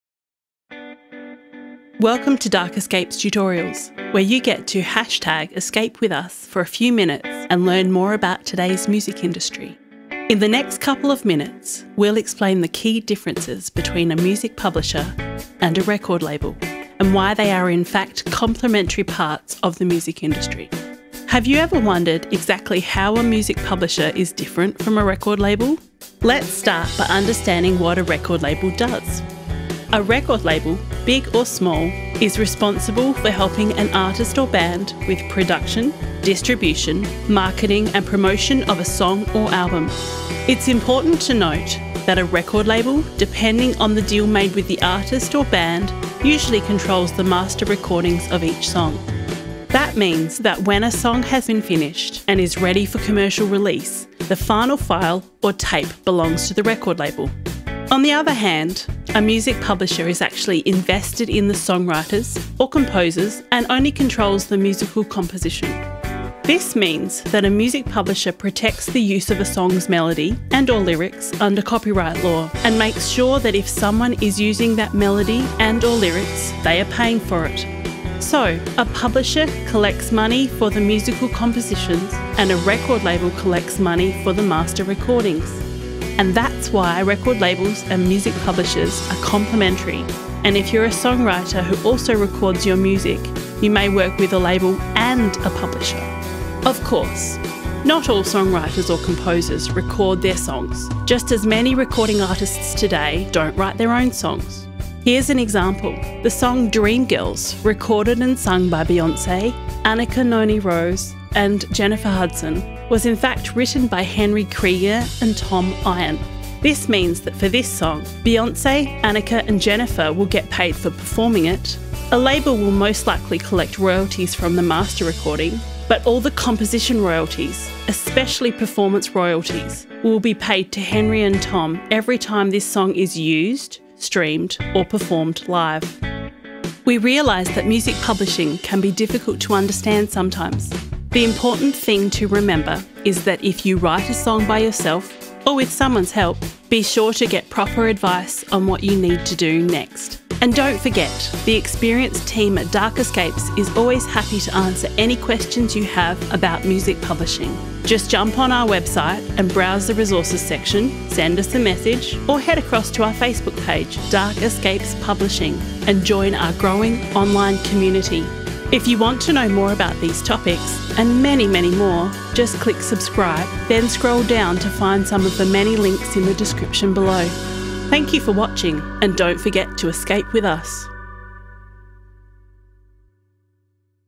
Dark Escapes Publishing: How Does a Music Publisher Differ From a Record Label?
The Dark Escapes team present this brief educational video explaining the difference between a MusicPublisher & a RecordLabel to help songwriters and artists better understand aspects of the music industry.

Remember to click LIKE or FOLLOW, and ESCAPEWITHUS

CREDITS
- Video created using Doodly - production by Dark Escapes Media

- Voice over: Rosalie Miller

- Mixed & Mastered by Ian Miller at Melville Recording Studio